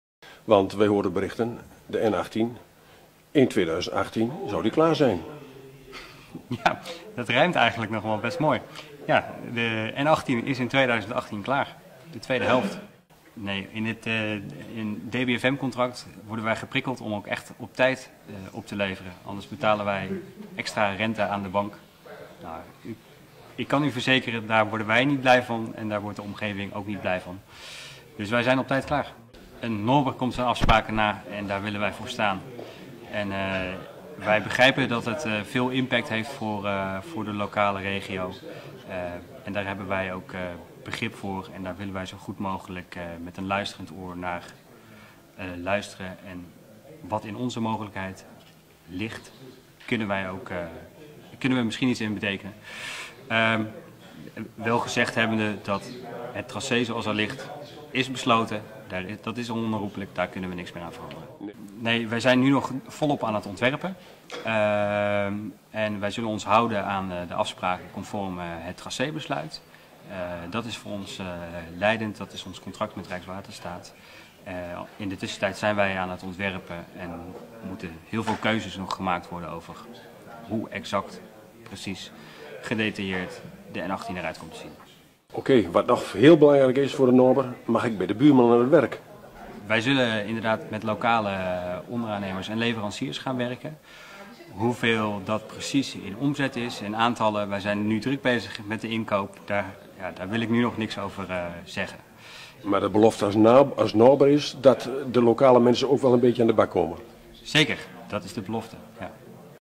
N18 Twenteroute Film
Projectmanager Rudolf van der Meulen verzekert dat Noaber18 lokale onderaannemers contracteert voor de aanleg van de nieuwe N18 Twenteroute.
Beeldverslagen over de voorbereiding en de aanleg van de nieuwe N18 Twenteroute, gemaakt door de filmclubs in Doetinchem, Groenlo, Berkelland, Haaksbergen en Enschede.